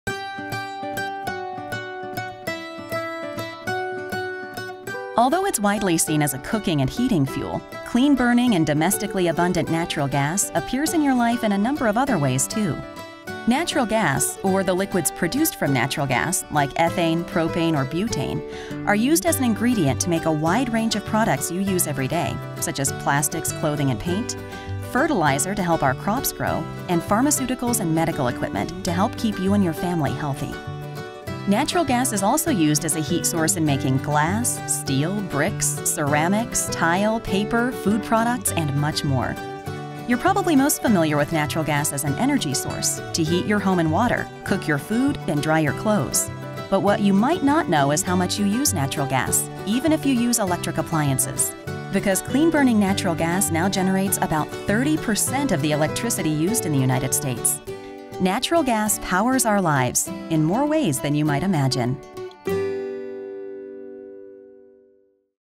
What products are made with natural gas?
Clean-burning and domestically-abundant natural gas powers our lives in more ways than you might imagine – as an ingredient in every day products, as a heat source for other products, and increasingly to generate electricity.